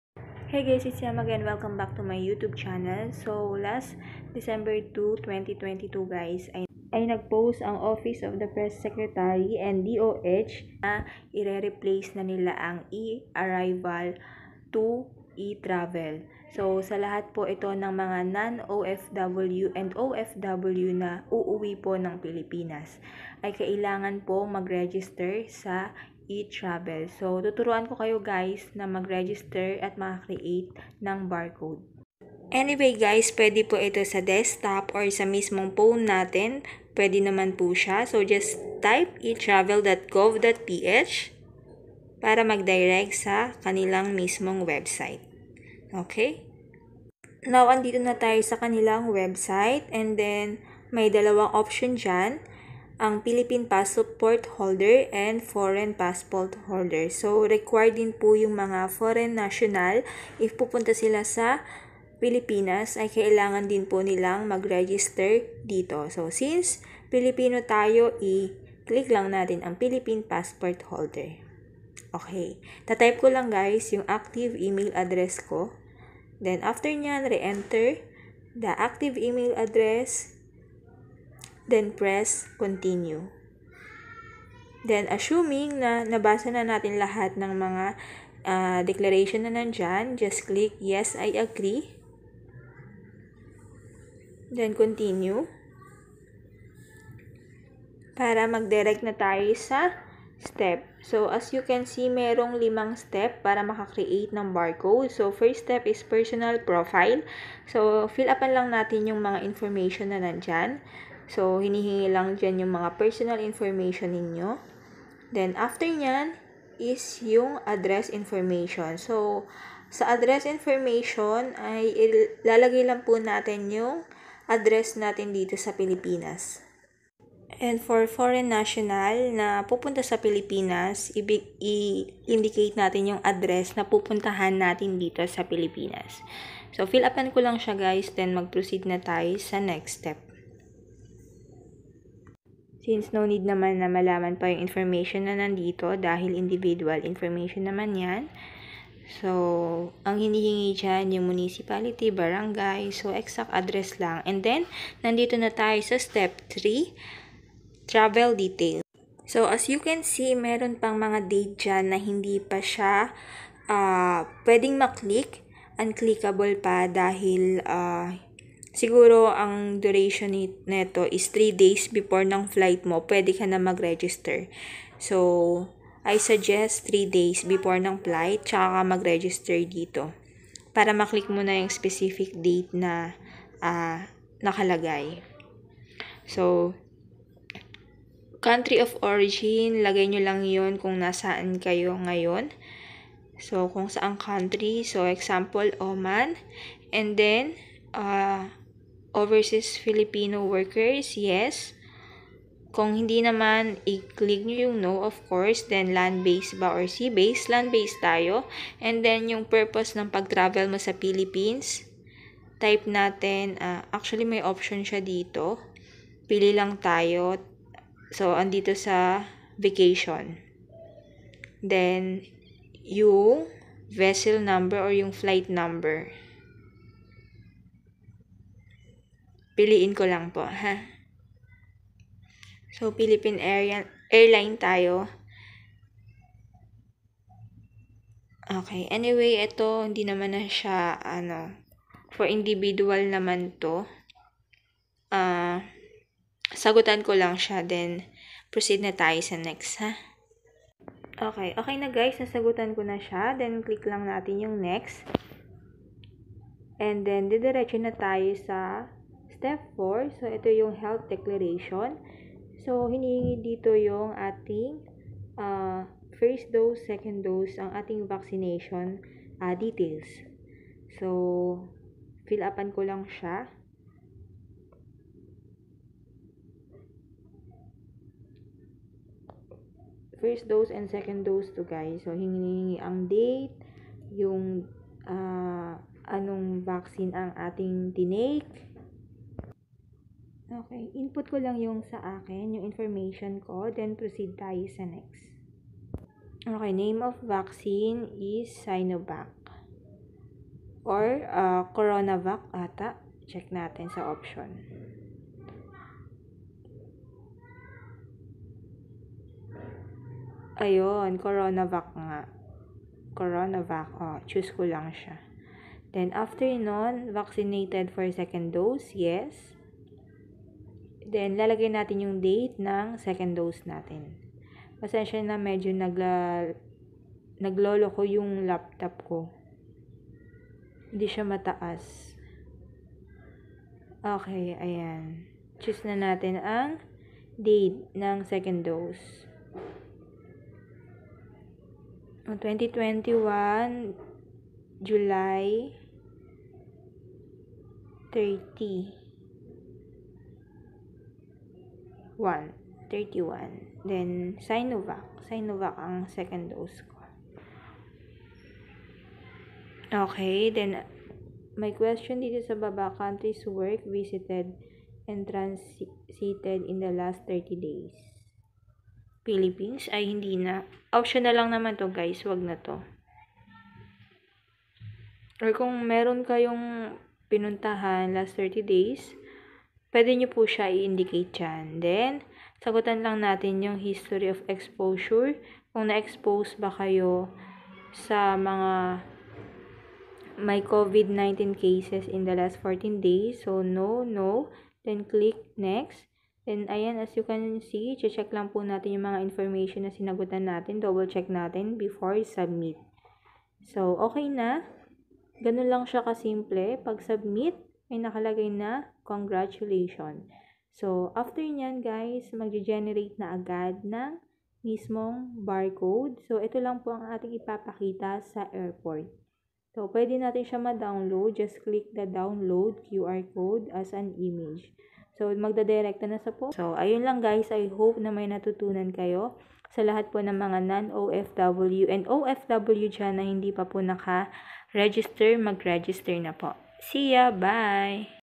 ETRAVEL CARD ONLINE REGISTRATION
Magtatravel ka ba o balik-bayan? Pwede nang mag-submit ng Electronic Travel Declaration sa pamamagitan lang ng tatlong steps!

1. Pumunta sa etravel.gov.ph o i-scan ang QR Code.

2. Sagutan ang personal profile, travel details, at health declaration.

3. I-download ang QR Code na ibibigay matapos magfill-up ng online form.

𝗡𝗢𝗧𝗜𝗖𝗘 𝗧𝗢 𝗣𝗨𝗕𝗟𝗜𝗖

𝗚𝗼𝘃𝗲𝗿𝗻𝗺𝗲𝗻𝘁 𝗜𝗻𝘁𝗲𝗴𝗿𝗮𝘁𝗲𝘀 𝗤𝘂𝗮𝗿𝗮𝗻𝘁𝗶𝗻𝗲 𝗮𝗻𝗱 𝗜𝗺𝗺𝗶𝗴𝗿𝗮𝘁𝗶𝗼𝗻 𝗔𝗿𝗿𝗶𝘃𝗮𝗹 𝗥𝗲𝗾𝘂𝗶𝗿𝗲𝗺𝗲𝗻𝘁𝘀 𝗳𝗼𝗿 𝗜𝗻𝘁𝗲𝗿𝗻𝗮𝘁𝗶𝗼𝗻𝗮𝗹 𝗣𝗮𝘀𝘀𝗲𝗻𝗴𝗲𝗿𝘀

Pursuant to the Philippine Government’s efforts to streamline its entry procedures and data collection processes, we inform you of the launch of the eTravel platform, an online registration system for travelers to the Philippines and returning residents.

Beginning 0001H of 02 December 2022, the Health Declaration Checklist of the Department of Health-Bureau of Quarantine may be accessed through etravel.gov.ph.

Passengers using the former will be redirected to the new eTravel domain. On December 5, the old domain of the One Health Pass will no longer be accessible to the public.

The eTravel platform will host the electronic version of the paper Arrival Card that travelers must submit upon arrival in the Philippines for immigration clearance. Starting 05 December 2022, the Bureau of Immigration will discontinue using the paper Arrival Card. All travelers will be required to provide their arrival information and health declaration through the eTravel portal.

Registration on the eTravel platform is free. The public is warned against transacting with any entities or websites soliciting payments and fees of any kind as these are in no way related to the official eTravel platform of the Philippine Government.

The eTravel platform is a joint initiative of the Department of Information and Communications Technology (DICT) in partnership with the Bureau of Immigration (BI), Bureau of Quarantine (BOQ), Bureau of Customs (BOC), Department of Tourism (DOT), Department of Health (DOH), Department of Justice (DOJ), and Department of Transportation (DOTr).